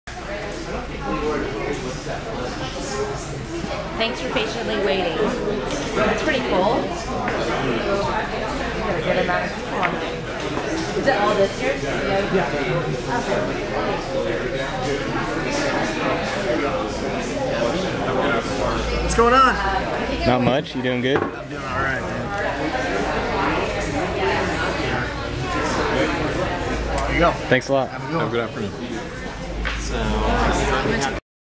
kole calhoun autograph signing
kole calhoun signed autographs at cox solution store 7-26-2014